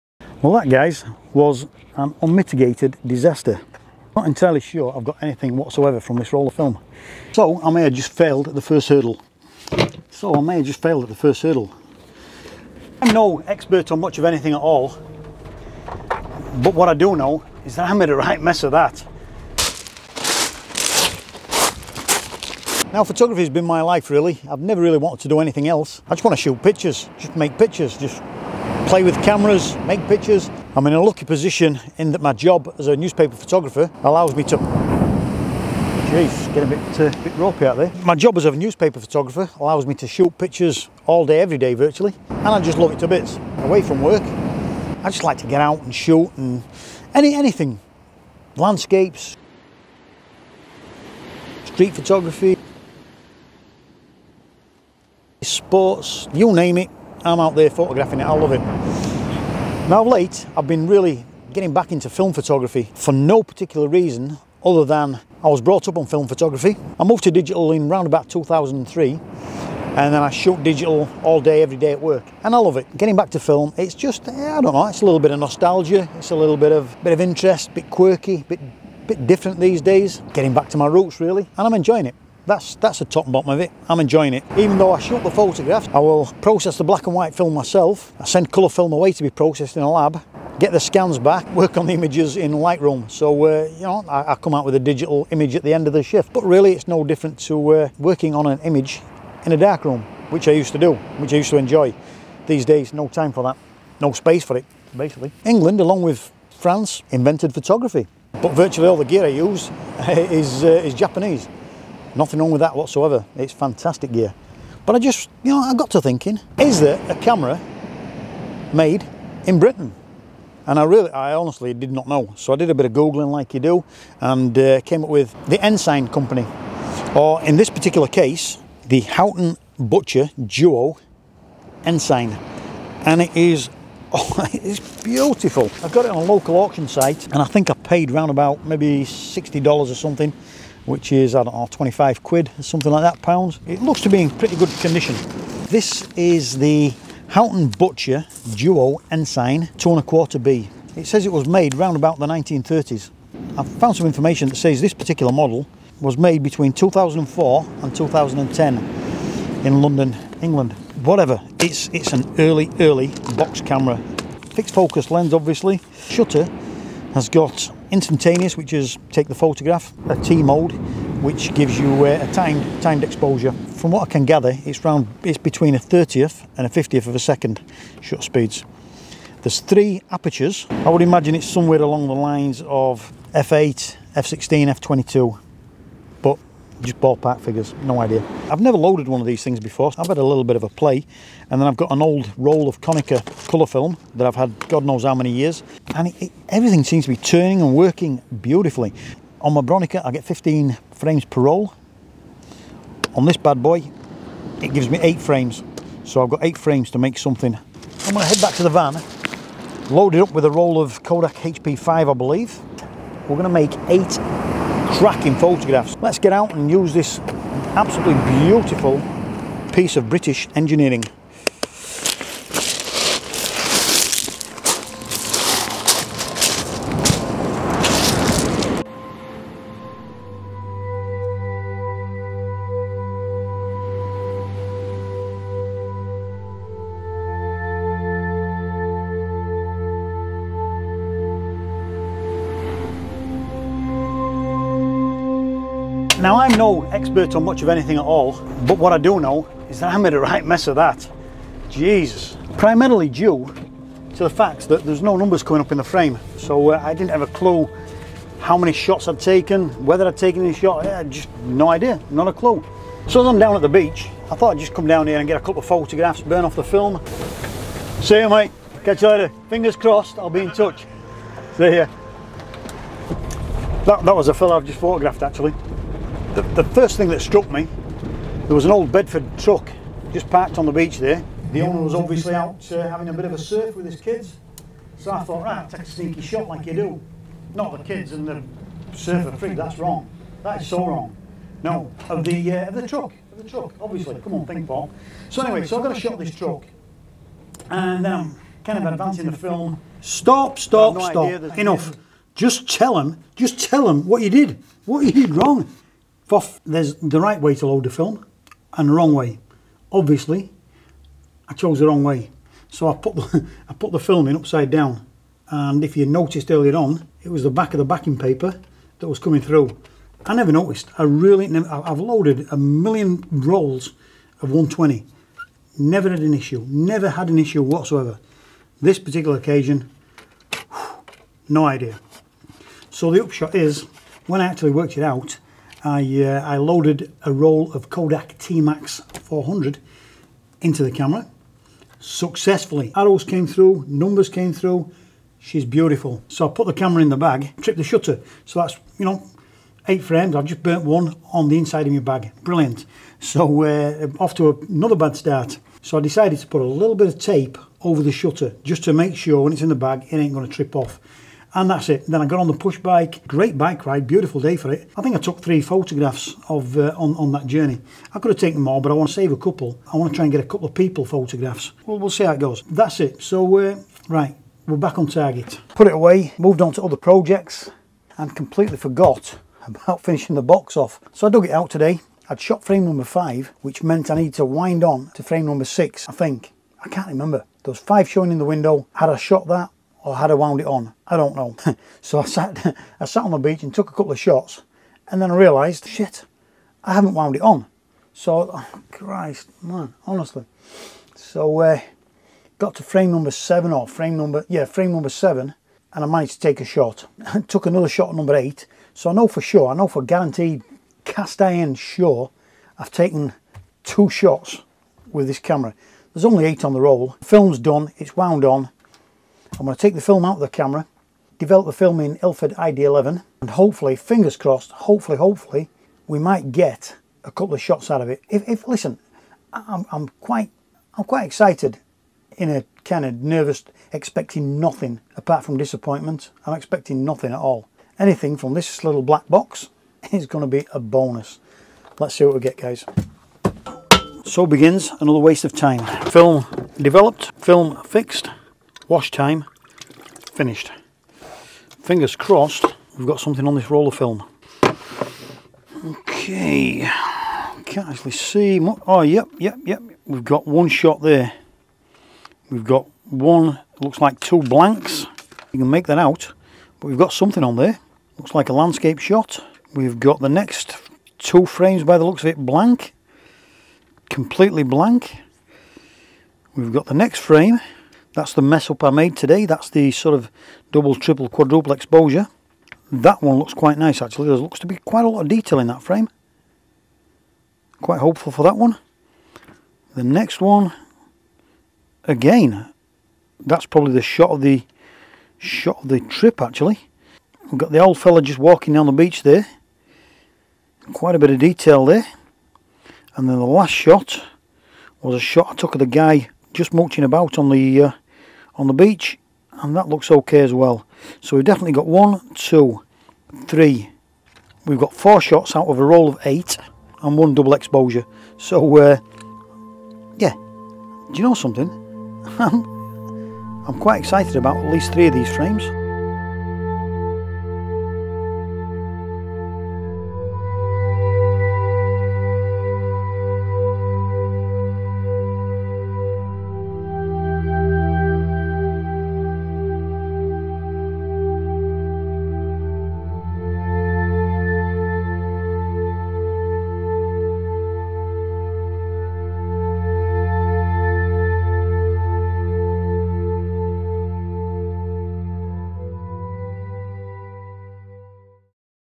BOX camera my first time
Film Photography | Vintage Camera
Vintage Camera first experience
Box Camera Photography. First Time using a Box Camera
Houghton Butcher Duo Ensign
So film photography here we go again trying an old, a very old Box Camera. These things were made to be easy to use, they clearly didn't factor in how fecking stupid I am.